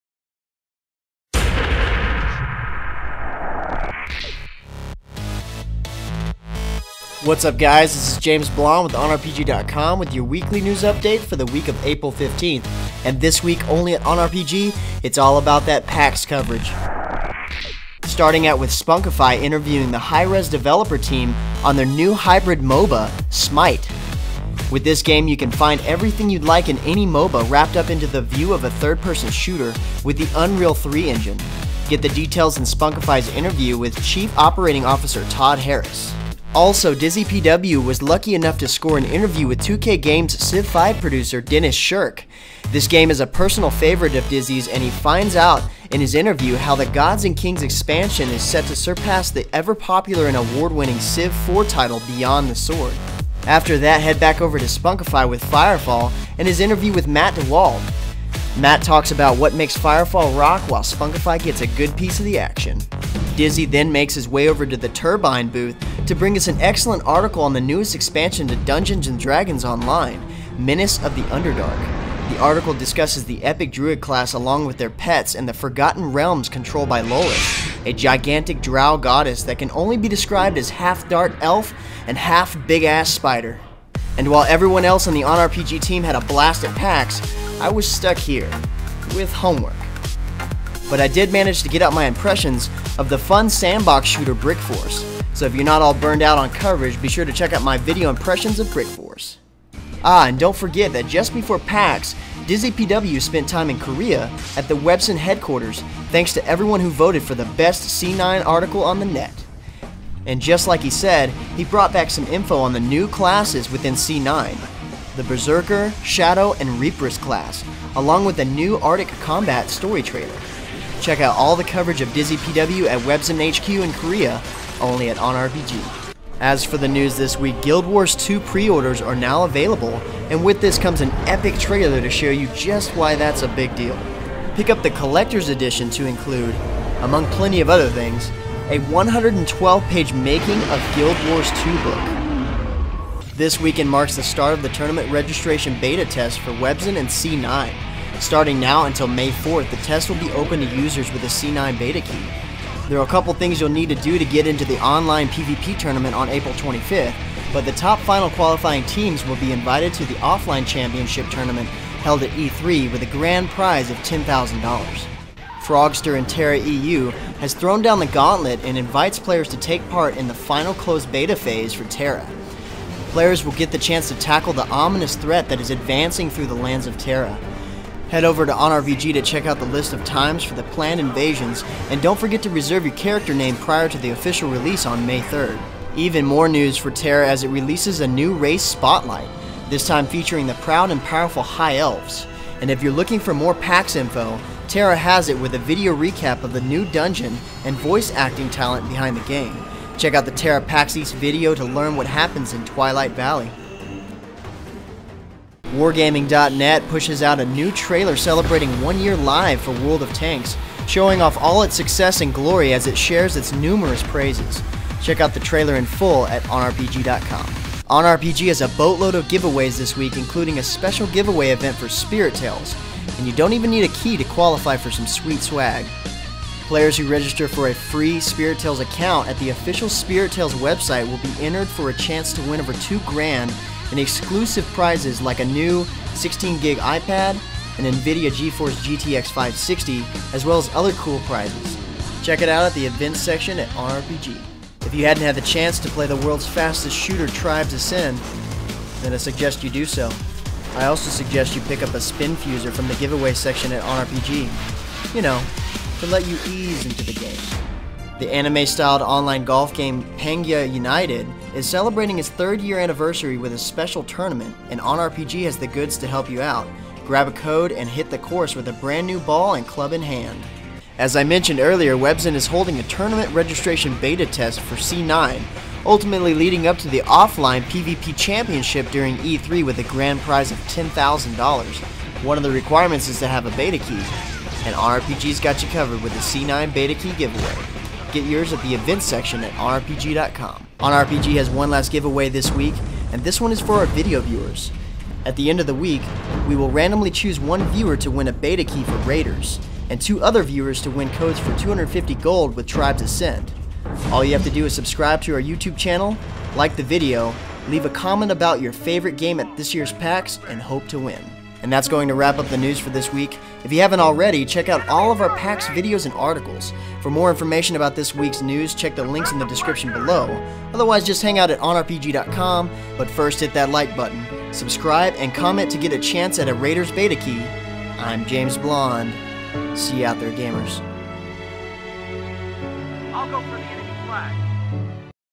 OnRPG News Update April 15th. w/ JamesBl0nde
Only @ OnRPG:

Smite Interview

Smite @ PAX

Civ 5 Gods and Kings interview

Civ 5 Gods and Kings Expansion @ PAX

FireFall interview

Firefall @ PAX

Menace of the Underdark

DDO Menace of Underdark @ PAX

News

Guild Wars 2 Pre-Orders available now

C9 PvP Tournament Beta Begins this weekend

Frogster Last CB Test for TERA EU

TERA Recaps PAX, CB, and showcases the High Elves

World of Tanks Turns 1 Year Old